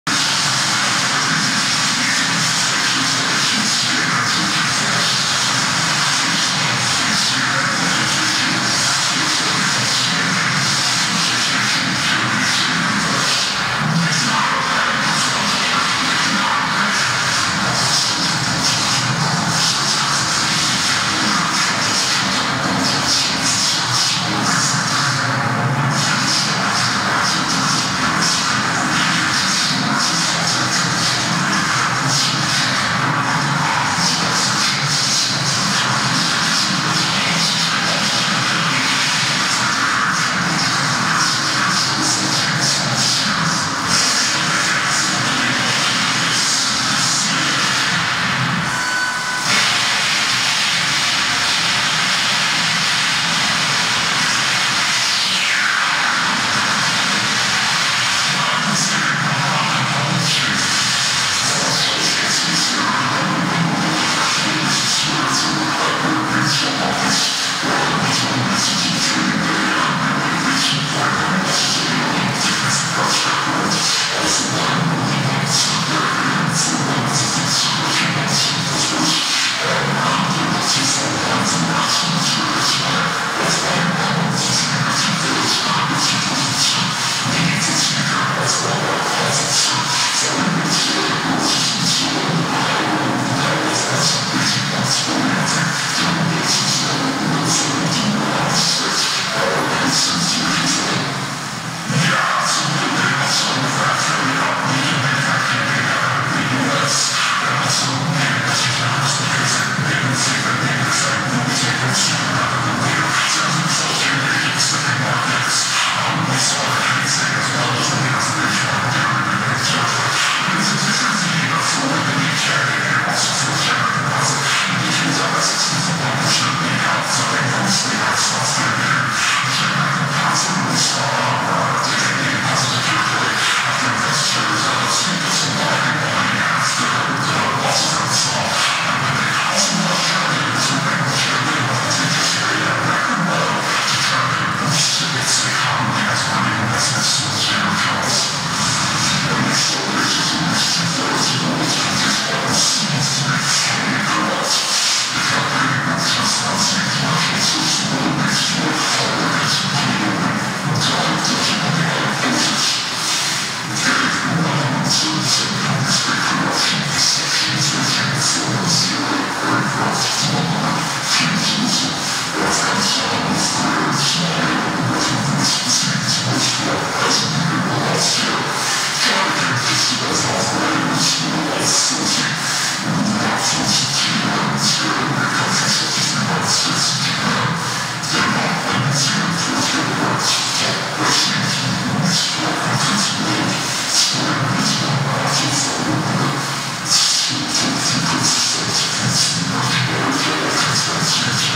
Supreme news live (PIXEL) frontview
Supreme news live je minimalistická instalace - projekce aplikace aktivní v reálném čase jež reflektuje reprezentaci světa prostřednictvím vysílaných zpráv a způsob jakým jsou vnímány. Projekce využívá konkrétní non-stop vysílání jako generativní základ pro vytvoření vizuálního uměleckého díla. Fenomén non-stop zpráv je uvažován jako ztěží adekvátní redukce aktuálního dění ve světě. Toto vysílání, tento redukovaný tok informací je v této práci dále redukován. Jedná se o formálně jednoduchou digitální redukci jež faktický obsah vysílání redukuje na nulu. Obsahová prázdnota nutí diváka opravdu přemýšlet co se v reálném světě děje, co onen jediný pixel - specifická barva reprezentuje, namísto běžné lhostejné konzumace informací.